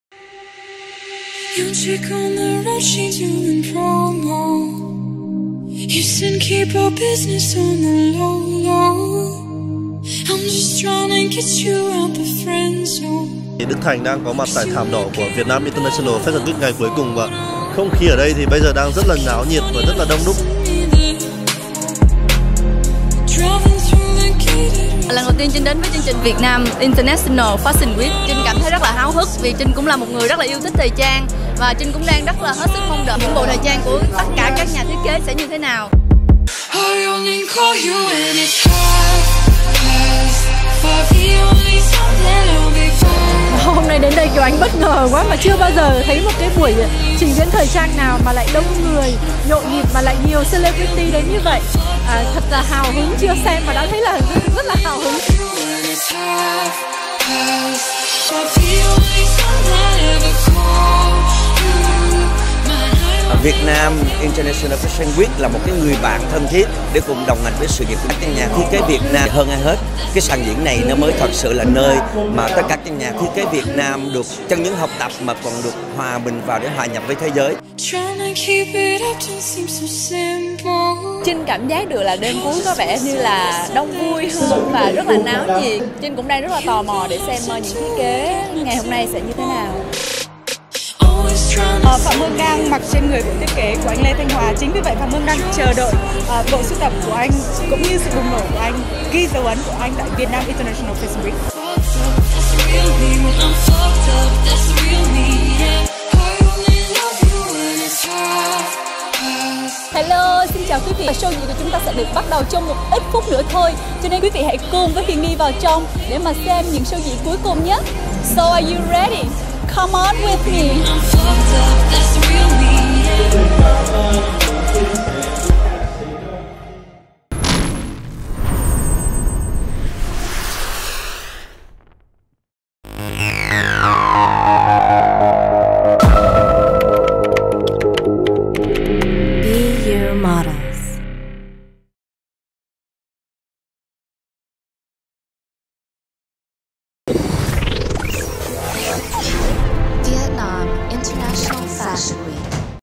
Angela Phương Trinh diện cây hàng hiệu hơn 1 tỷ tại thảm đỏ bế mạc || M36 Tv , F W 2016
By : Vietnam International Fashion Week